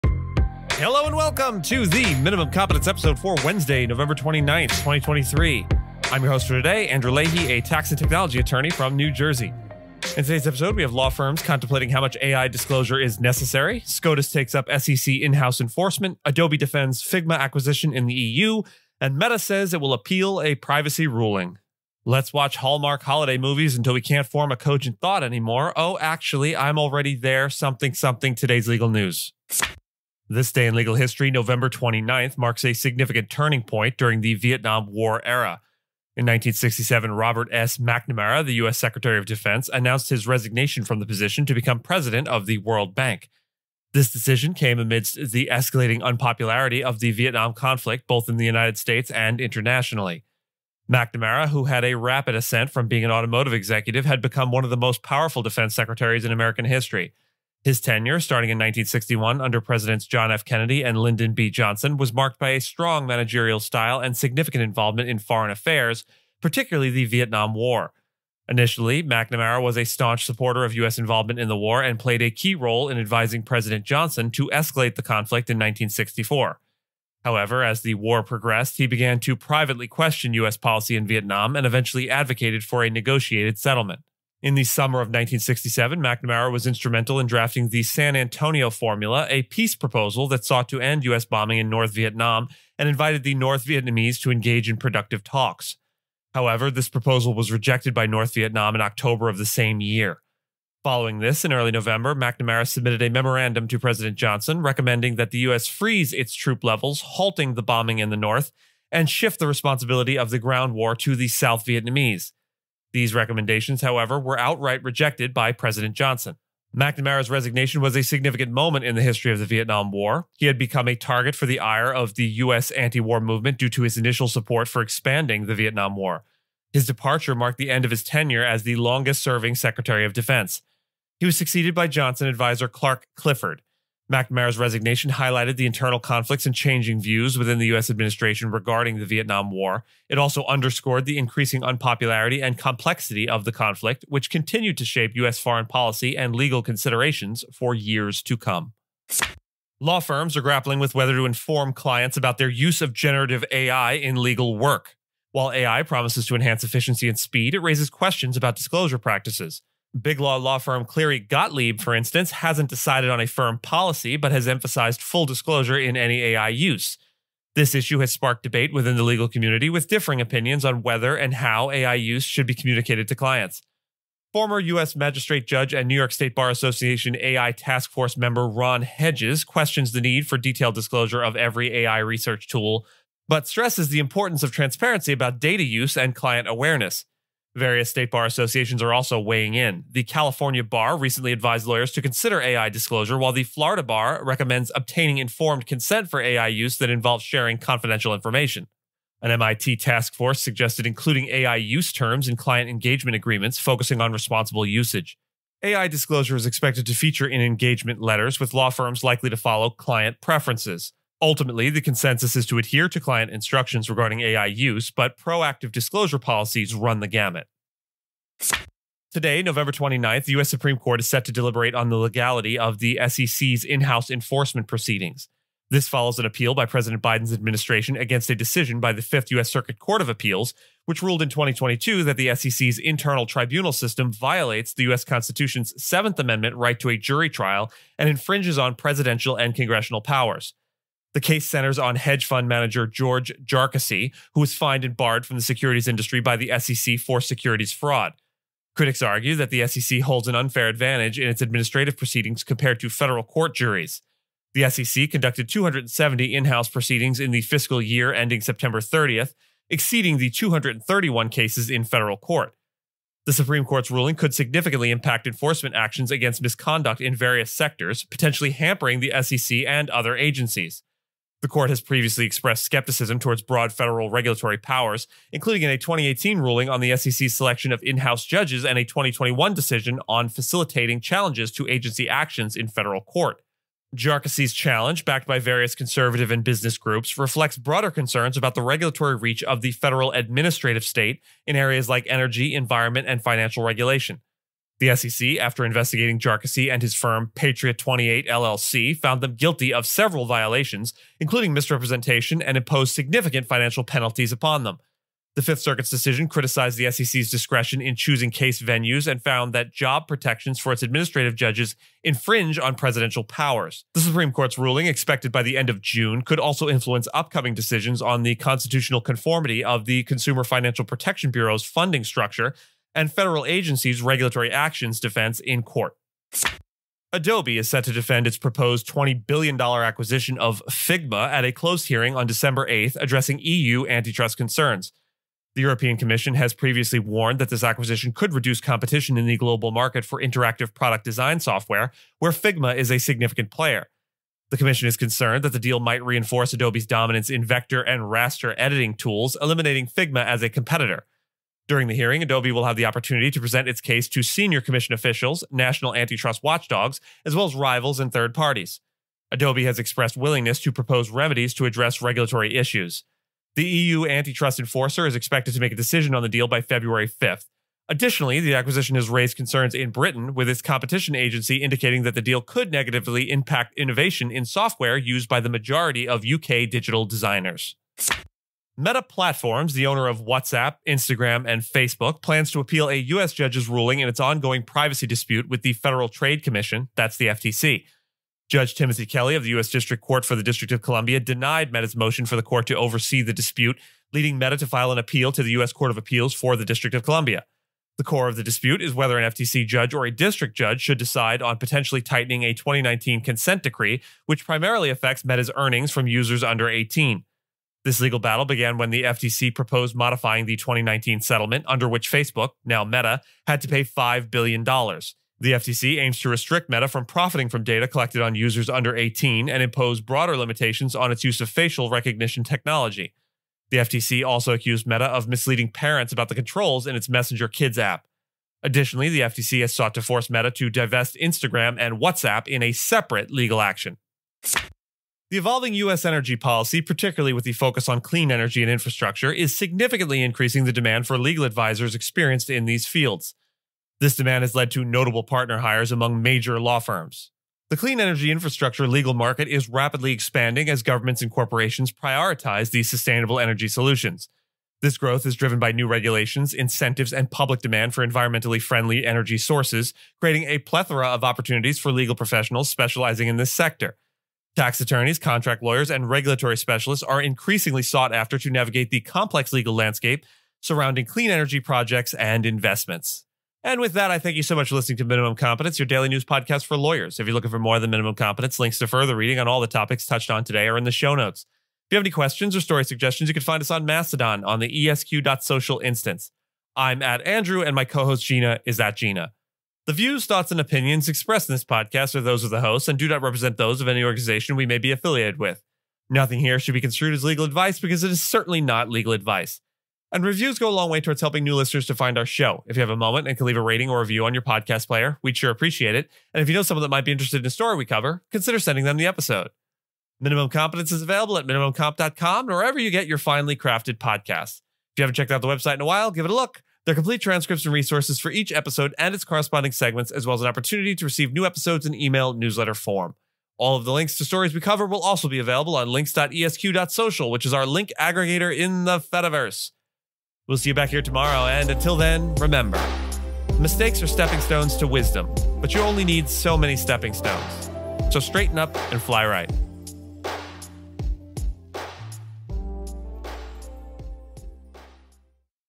Legal News for Weds 11/29 - AI Disclosure by Law Firms, SCOTUS Looks at SEC In-House Enforcement,...
On this day in legal history, November 29 marks a significant turning point during the Vietnam War era. In 1967, Robert S. McNamara, the U.S. Secretary of Defense, announced his resignation from the position to become president of the World Bank. This decision came amidst the escalating unpopularity of the Vietnam Conflict both in the United States and internationally.

McNamara, who had a rapid ascent from being an automotive executive, had become one of the most powerful Defense Secretaries in American history. His tenure, starting in 1961 under Presidents John F. Kennedy and Lyndon B. Johnson, was marked by a strong managerial style and significant involvement in foreign affairs, particularly the Vietnam War. Initially, McNamara was a staunch supporter of U.S. involvement in the war and played a key role in advising President Johnson to escalate the conflict in 1964. However, as the war progressed, he began to privately question U.S. policy in Vietnam and eventually advocated for a negotiated settlement.

In the summer of 1967, McNamara was instrumental in drafting the San Antonio formula, a peace proposal that sought to end U.S. bombing in North Vietnam and invited the North Vietnamese to engage in productive talks. However, this proposal was rejected by North Vietnam in October of the same year. Following this, in early November, McNamara submitted a memorandum to President Johnson, recommending that the U.S. freeze its troop levels, halt the bombing in the north, and shift the responsibility of the ground war to the South Vietnamese. These recommendations, however, were outright rejected by President Johnson.

McNamara’s resignation was a significant moment in the history of the Vietnam War. He had become a target for the ire of the U.S. anti-war movement due to his initial support for expanding the Vietnam War. His departure marked the end of his tenure as the longest-serving Secretary of Defense. He was succeeded by Johnson adviser Clark Clifford. McNamara's resignation highlighted the internal conflicts and changing views within the U.S. administration regarding the Vietnam War. It also underscored the increasing unpopularity and complexity of the conflict, which continued to shape U.S. foreign policy and legal considerations for years to come.

Law firms are grappling with whether to inform clients about their use of generative artificial intelligence (AI) in legal work. While AI promises to enhance efficiency and speed, it raises questions about disclosure practices. Cleary Gottlieb Steen & Hamilton, for instance, hasn't decided on a firm policy but emphasizes full disclosure in any AI use. This issue has sparked debate within the legal community, with differing opinions on whether and how AI use should be communicated to clients.

Former U.S. magistrate judge and New York State Bar Association AI task force member Ron Hedges questions the need for detailed disclosure of every AI research tool used but stresses the importance of transparency about data use and client awareness. Various state bar associations are also weighing in. The California Bar recently advised lawyers to consider AI disclosure, while the Florida Bar recommends obtaining informed consent for AI use that involves sharing confidential information.

AI disclosure is expected to feature in engagement letters, with law firms likely to follow client preferences. Ultimately, the consensus is to adhere to client instructions regarding AI use, but proactive disclosure policies run the gamut.

Law Firms Wrestle With How Much to Tell Clients About AI Use

On November 29, the U.S. Supreme Court is set to deliberate on the legality of the Securities and Exchange Commission's (SEC) in-house enforcement proceedings. This follows an appeal by President Biden's administration against a decision by the 5th U.S. Circuit Court of Appeals, which ruled in 2022 that the SEC’s internal tribunal system violates the U.S. Constitution's Seventh Amendment right to a jury trial and infringes on presidential and congressional powers.

The case centers on hedge fund manager George Jarkesy, who was fined and barred from the securities industry by the SEC for securities fraud. Critics argue that the SEC holds an unfair advantage in its administrative proceedings compared to federal court juries. The SEC conducted 270 in-house proceedings in the fiscal year ending September 30, exceeding the 231 cases in federal court.

The Supreme Court's ruling could significantly impact enforcement actions against misconduct in various sectors, potentially hampering the SEC and other agencies. The court has previously expressed skepticism towards broad federal regulatory powers, including in a 2018 ruling on the SEC's selection of in-house judges and a 2021 decision facilitating challenges to agency actions in federal court.

Jarkesy's challenge...